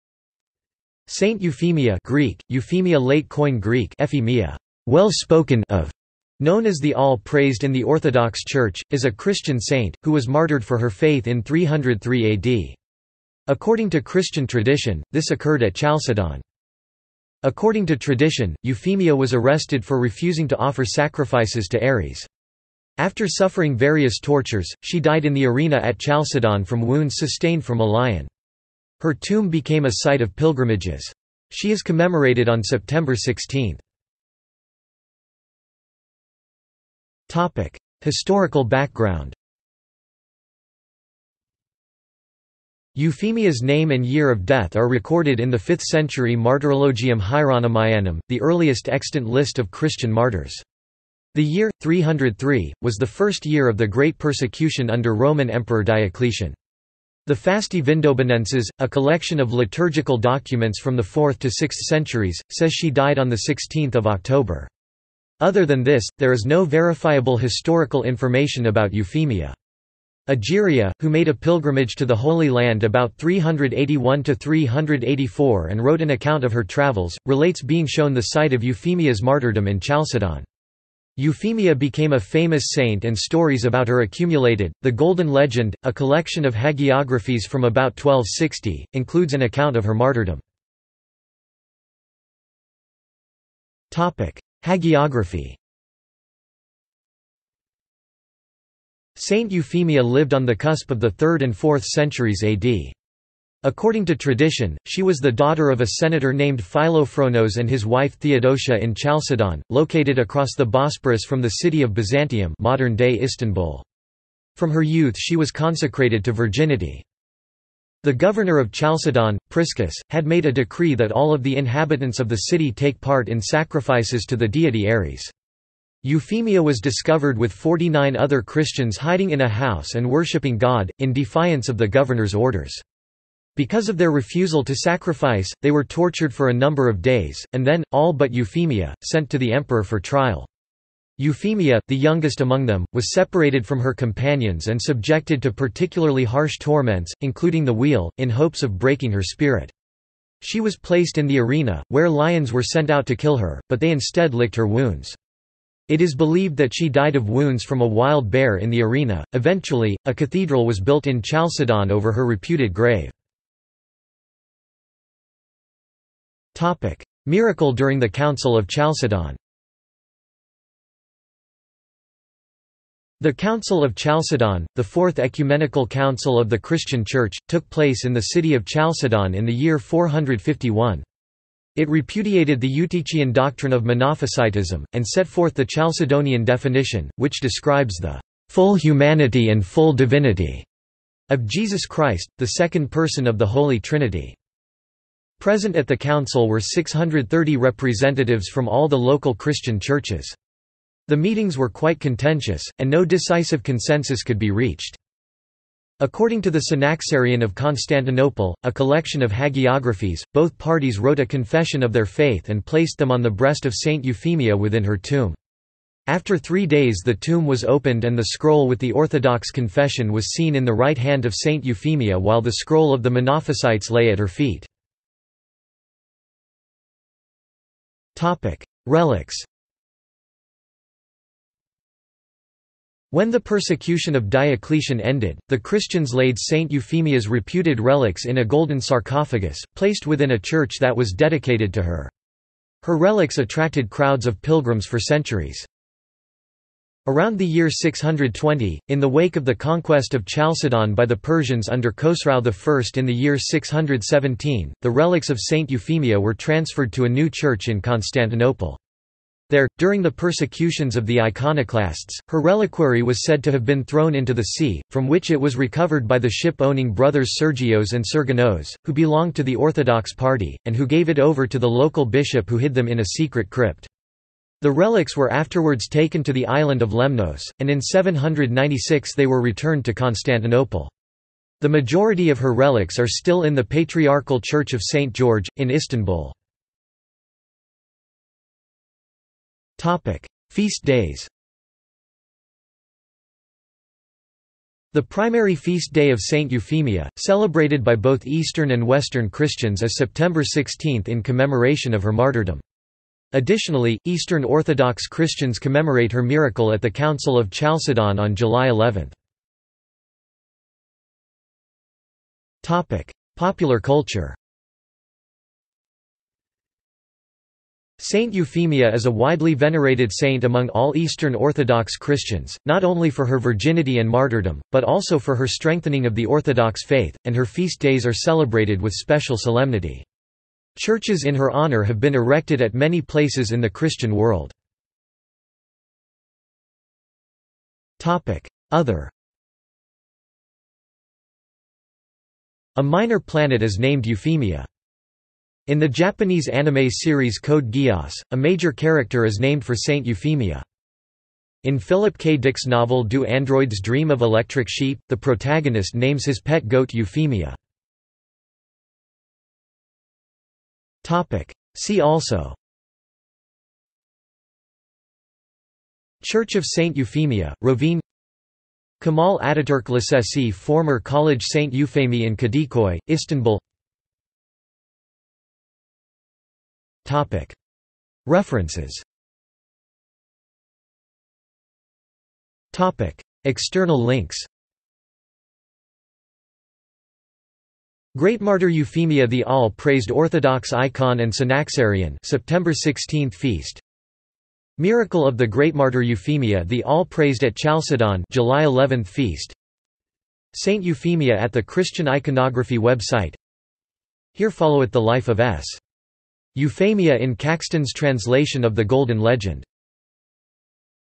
Euphemia | Wikipedia audio article
This is an audio version of the Wikipedia Article:
Euphemia

- Socrates

SUMMARY
Saint Euphemia (Greek: Ευφημία Late Koine Greek [efiˈmia]), "well-spoken [of]", known as the All-praised in the Orthodox Church, is a Christian saint, who was martyred for her faith in 303 AD. According to Christian tradition, this occurred at Chalcedon.
According to tradition, Euphemia  was arrested for refusing to offer sacrifices to Ares. After suffering various tortures, she died in the arena at Chalcedon from wounds sustained from a lion. Her tomb became a site of pilgrimages. She is commemorated on September 16.